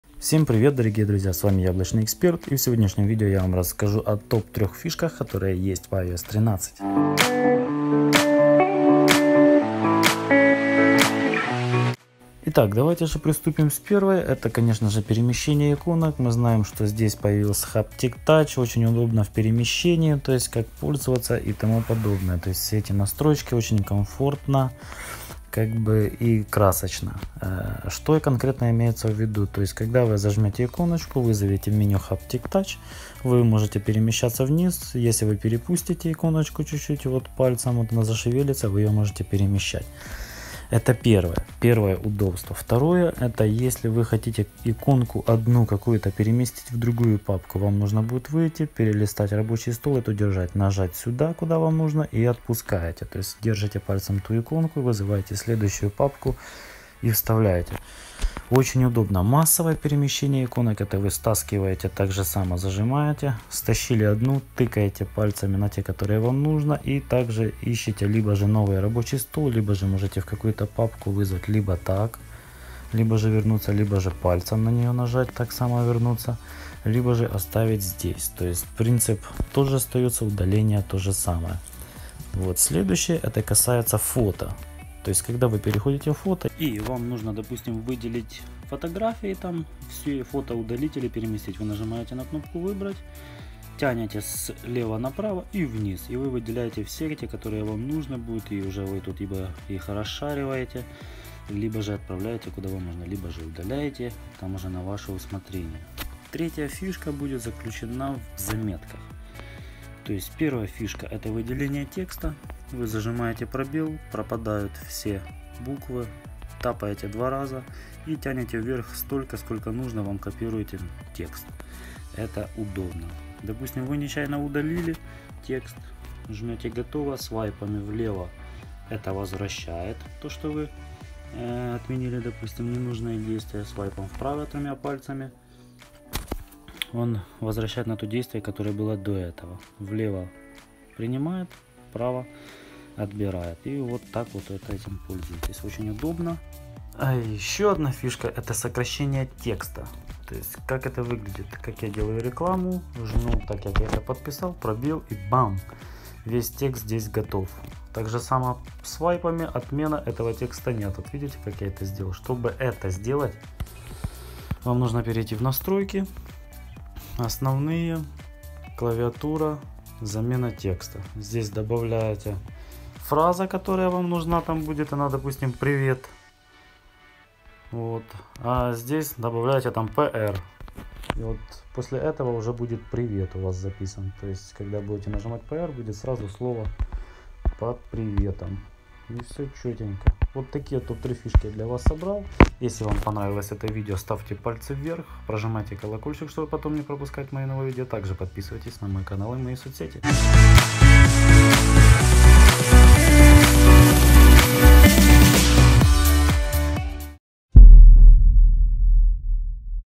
❕Лайфхаки iOS 13 о которых ты не знал❕- Apple Experts
Самые свежие лайфхаки с которыми вам станет легче в использовании вашего iPhone/iPad